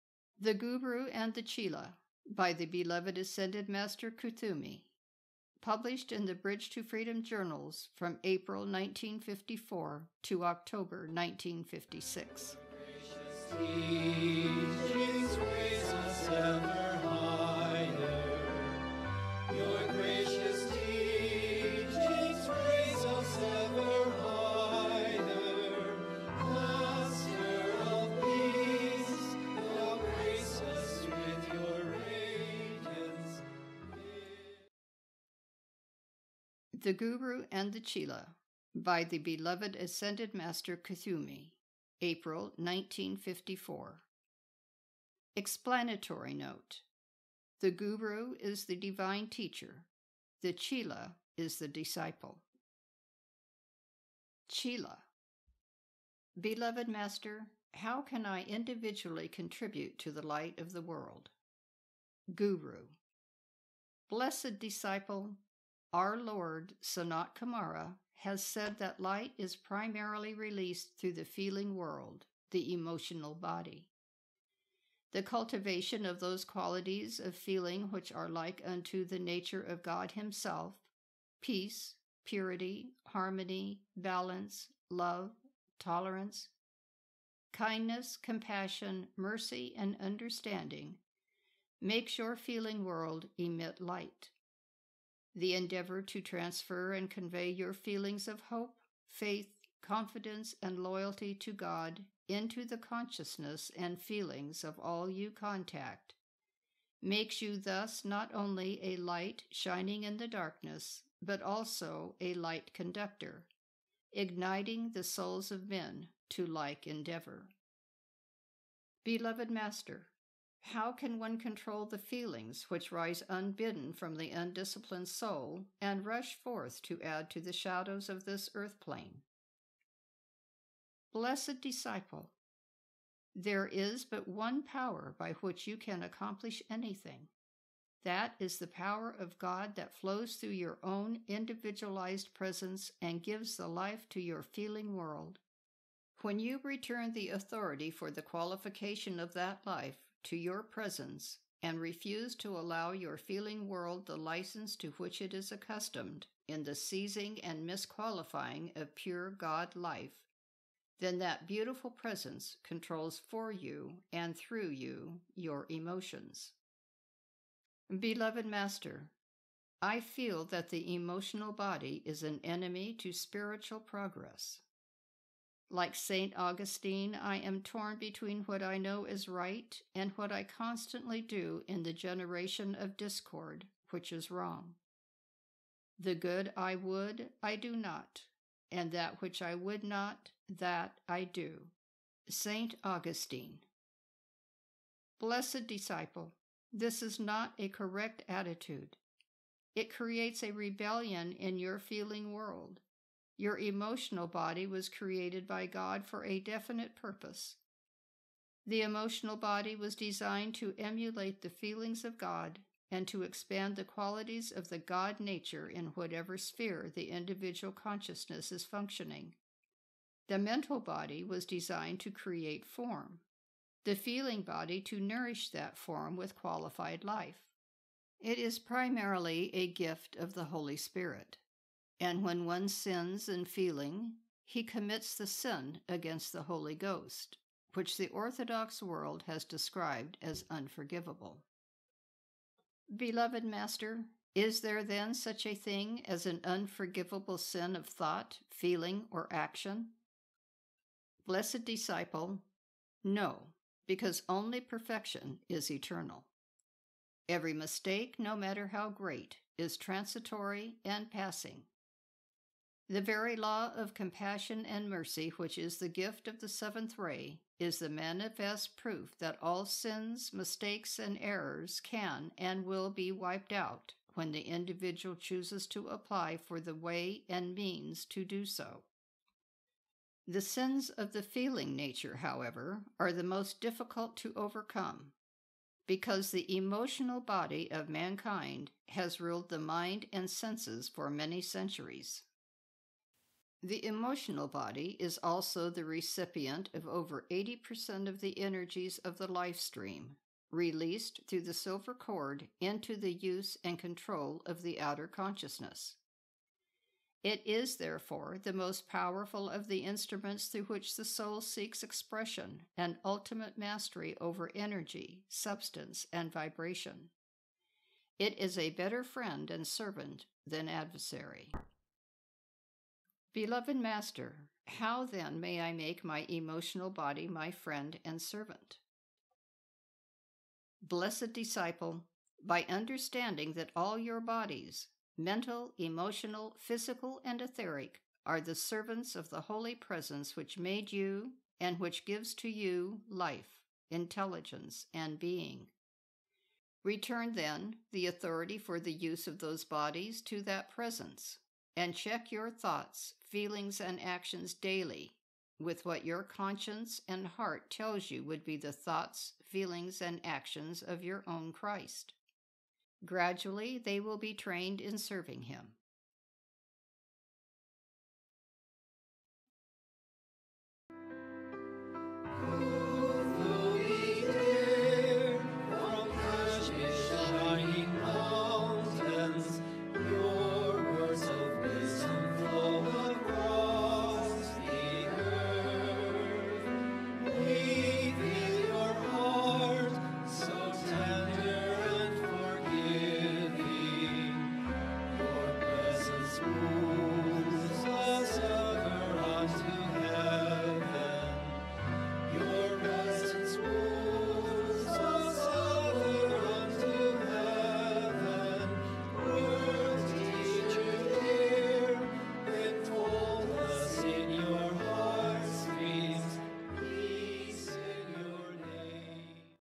Light is primarily released through the emotional body.
The Guru And the Chela. Audiobook, 1."Chela—Q. Beloved Master, how can I individually contribute to the Light of the World?
GURU—A. Blessed disciple! Our Lord, Sanat Kumara has said that light is primarily released through the feeling world (the emotional body). The cultivation of those qualities of feeling which are like unto the nature of God Himself, peace, purity, harmony, balance, love, tolerance, kindness, compassion, mercy and understanding makes your feeling world emit light. The endeavor to transfer and convey your feelings of hope, faith, confidence and loyalty to God into the consciousness and feelings of all you contact makes you thus not only a Light shining in the darkness, but also a Light conductor, igniting the souls of men to like endeavor.
Chela—Q. Beloved Master. How can one control the feelings, which rise unbidden from the undisciplined soul and rush forth to add to the shadows of this Earth plane?
Guru—A. Blessed disciple! There is but ONE POWER by which you can accomplish anything. That is the Power of God that flows through your own Individualized Presence and gives the life to your feeling world. When you return the authority for the qualification of that life to your Presence and refuse to allow your feeling world the license to which it is accustomed in the seizing and misqualifying of pure God life, then that Beautiful Presence controls for you and through you your emotions."
Ascended Master Kuthumi. "The Guru and the Chela". The Bridge to Freedom Journal. April, 1954.